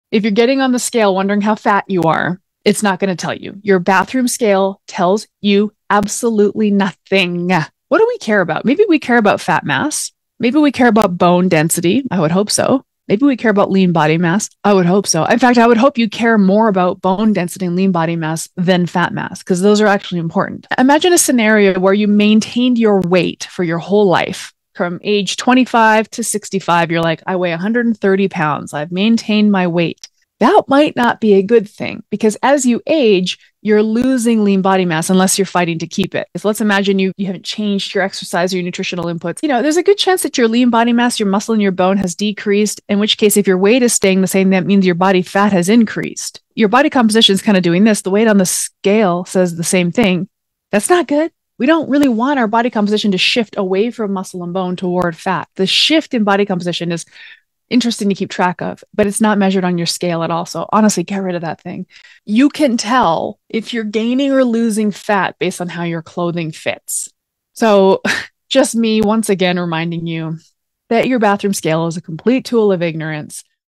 Scale weight is not the metric to care about. STOP IT.
If your weight goes down because your lean body mass went down: this is a BAD THING. The thing is, the bathroom scale doesn't tell you WHAT you're losing. It is not a useful measure of body fat. Smash it with a sledgehammer. It's a tool of ignorance.